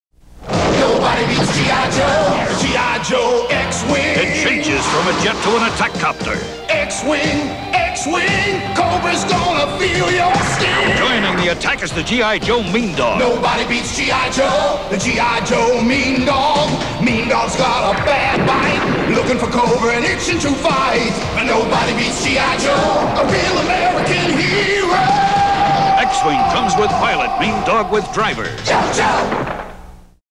GIJoe 1988 TV Commercial 13: X-Wing and Mean Dog - from Griffin Bacal Inc VHS Master 1080p HD
Griffin Bacal Inc. was a global advertising agency founded in 1978 by Tom Griffin and Joe Bacal. Hasbro partnered with Griffin Bacal to create advertising campaigns for G.I.Joe: A Real American Hero, Transformers, and more. This video was digitized from a Griffin Bacal Inc. VHS master dated 3/2/88 and labeled "G.I.Joe 1988 Campaign."